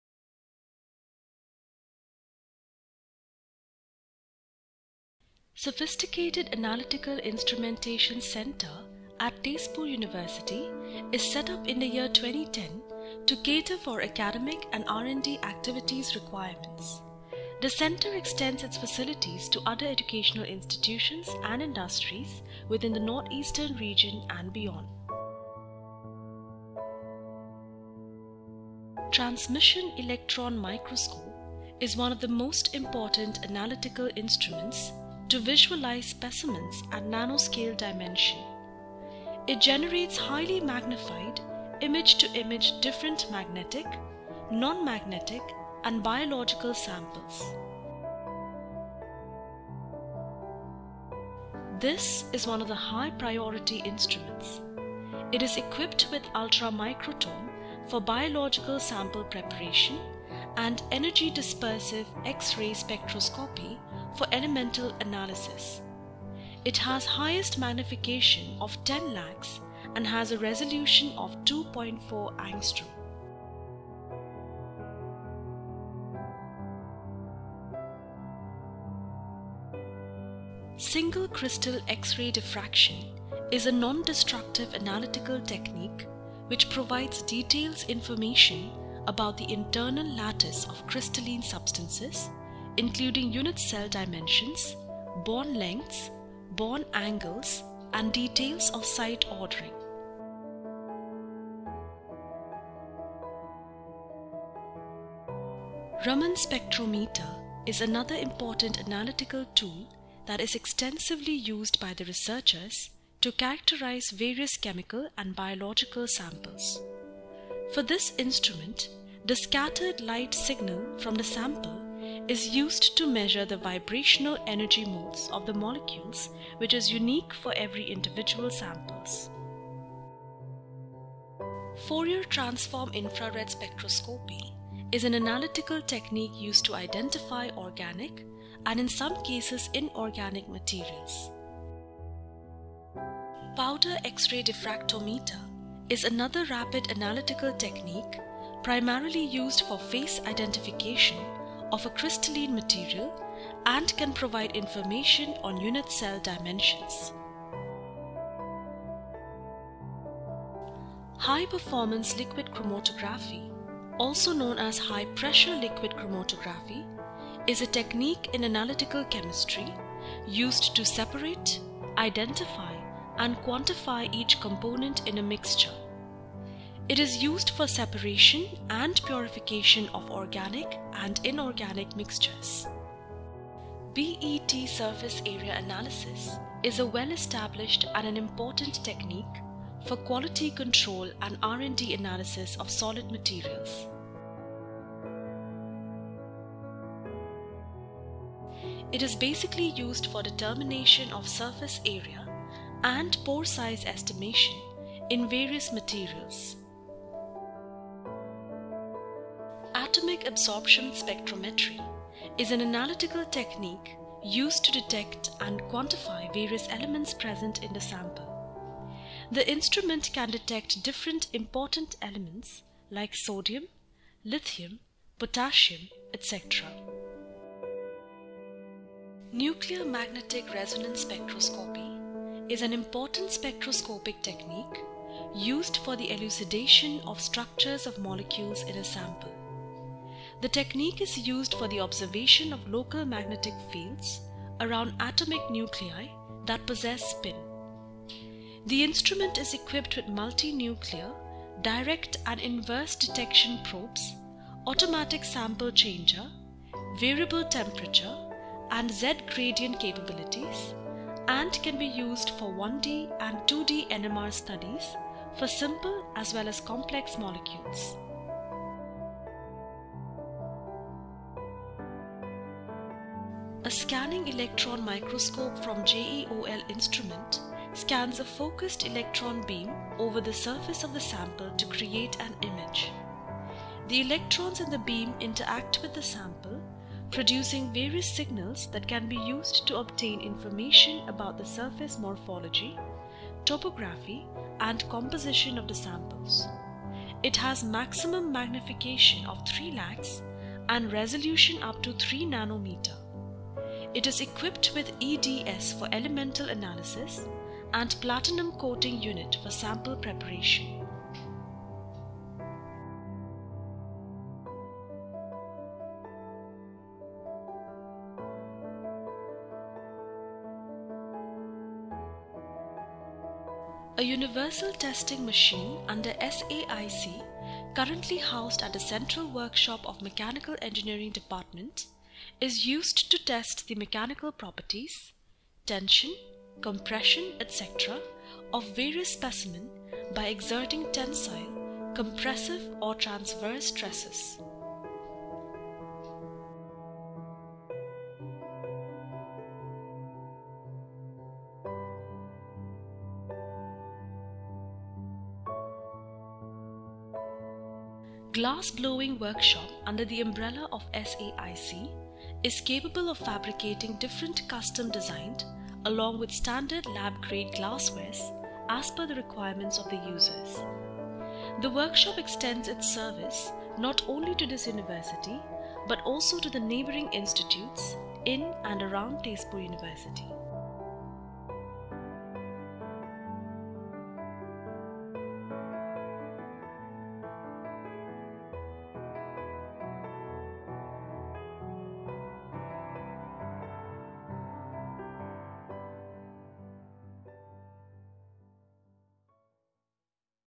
Sophisticated Analytical Instrumentation Centre (SAIC) || Tezpur University
The Sophisticated Analytical Instrumentation Centre (SAIC) was established at Tezpur University in 2010 to cater to sophisticated analytical equipment needs in R&D.

Currently, twenty-five pieces of equipment including a Transmission electron Microscope (TEM), Scanning Electron microscope (SEM), Single crystal XRD, Powder XRD, BET (Surface and Pore size analyser), Raman Spectrometer, Fourier Transform Infrared Spectrometer (FTIR) etc. are housed and maintained by SAIC.

The Centre extends these facilities to internal users as well as other educational institutions and industries within the northeast region and beyond, as many do not have the resources to procure and maintain such sophisticated instruments.